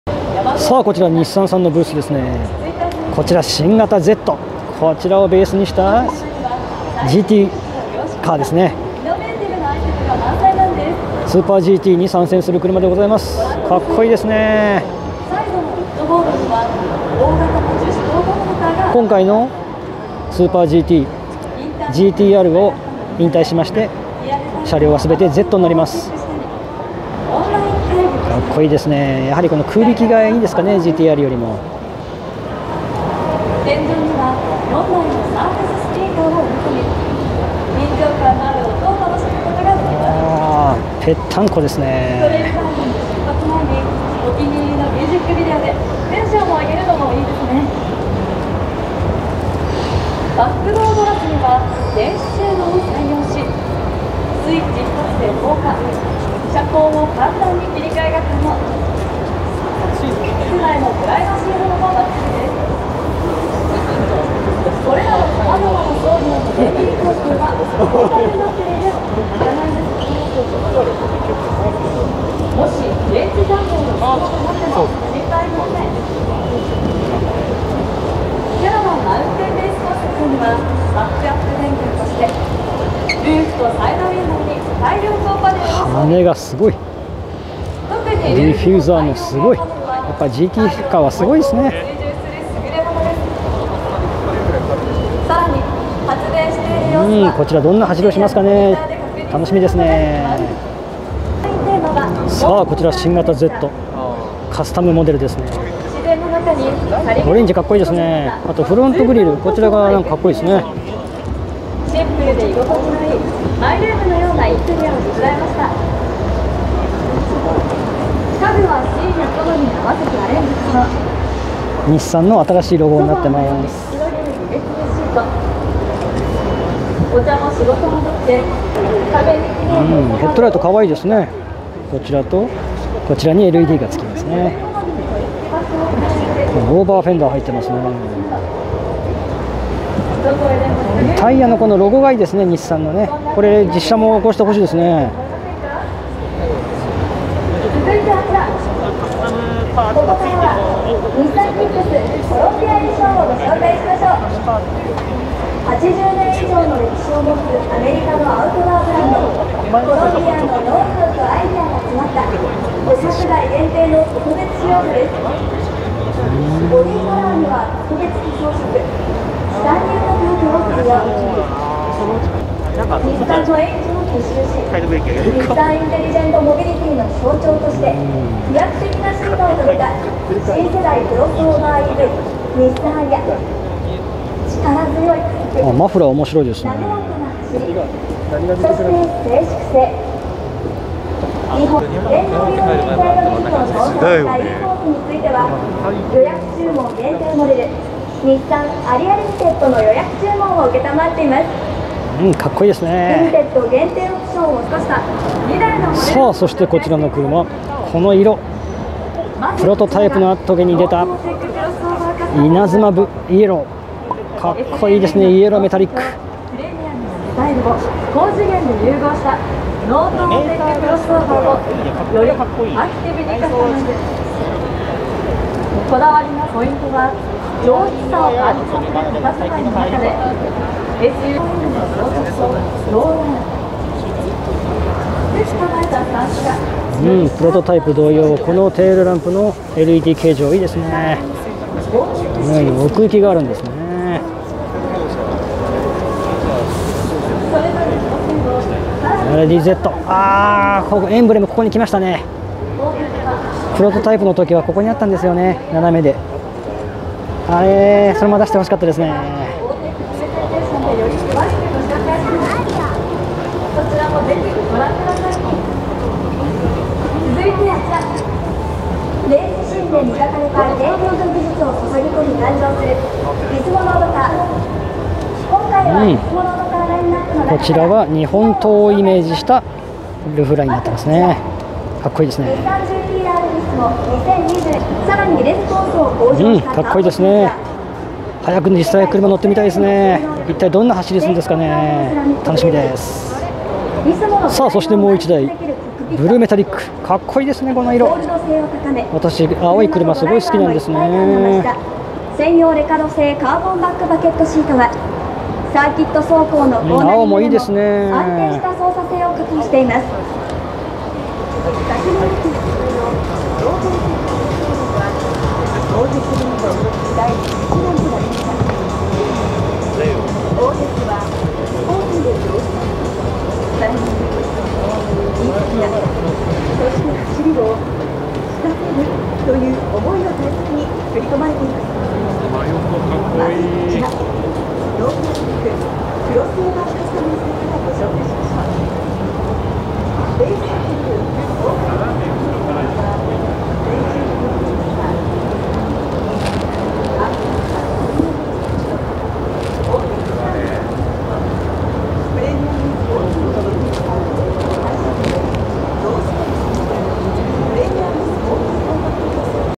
東京オートサロン2022 現地リポート 4 TOKYO AUTO SALON REPORT NISSAN FEARLADY Z
東京オートサロン2022 日産ブースからの現地リポートです。新型フェアレディZがついに発表されました。